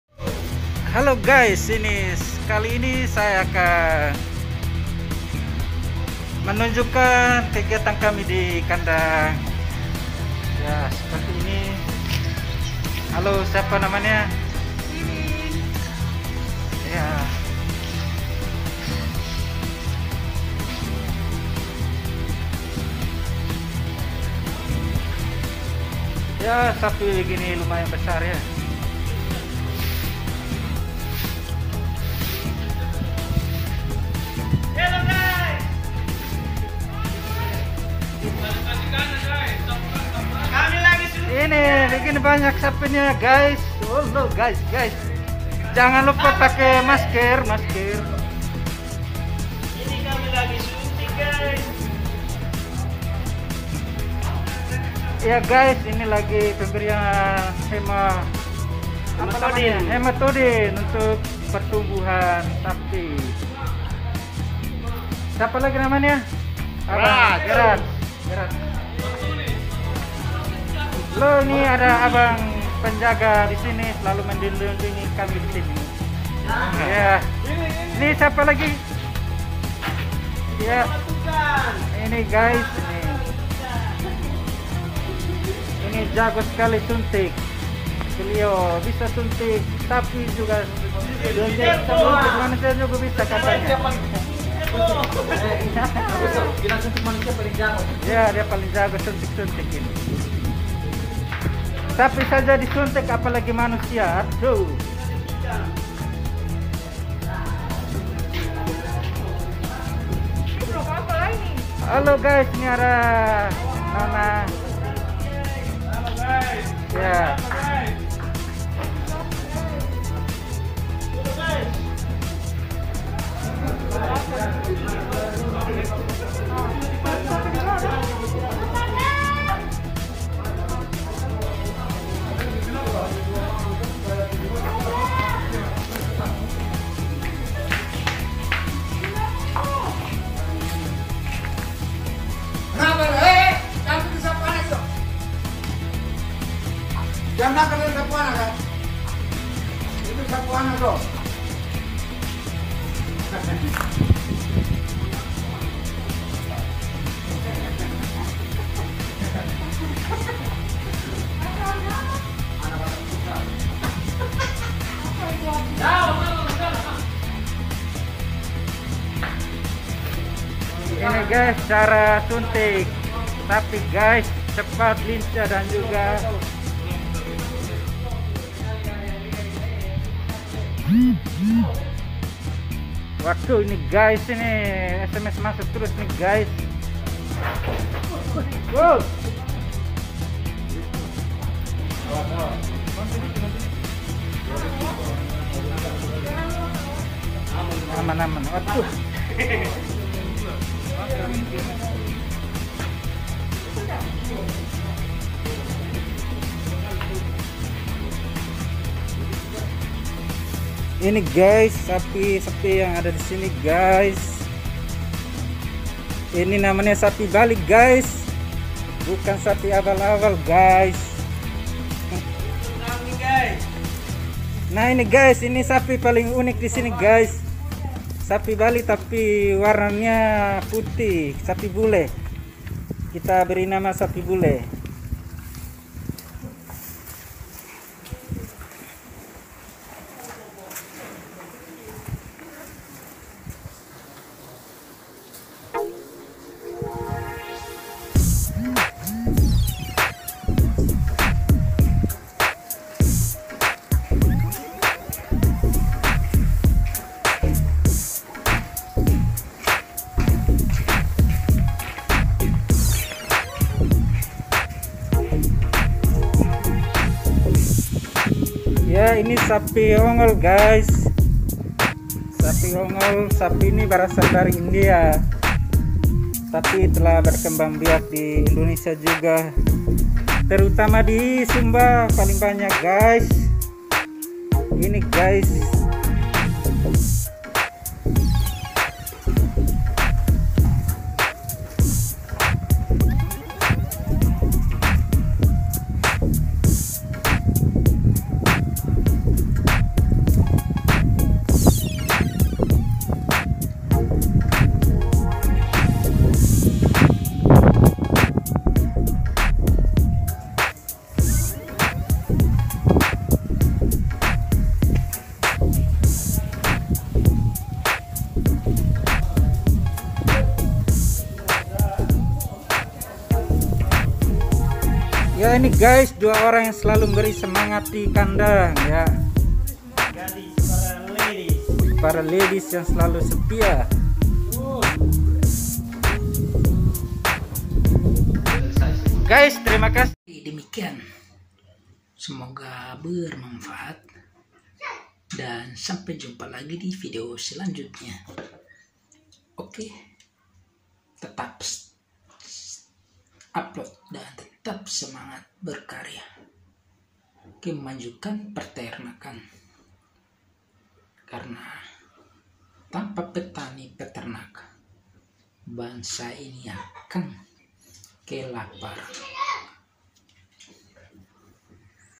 Penyuntikan Hematodin pada Sapi: Proses dan Manfaatnya
Dalam video ini, kami akan membahas proses penyuntikan Hematodin pada sapi dan manfaatnya bagi kesehatan ternak. Hematodin adalah obat yang sering digunakan untuk meningkatkan kualitas darah dan membantu pemulihan setelah penyakit atau cedera. Kami akan menunjukkan langkah-langkah yang tepat dalam memberikan suntikan Hematodin, termasuk dosis yang disarankan dan tips keamanan saat melakukan prosedur ini.

Tonton video ini sampai selesai untuk mengetahui bagaimana Hematodin bisa meningkatkan daya tahan tubuh dan kesehatan ternak Anda!

Tagar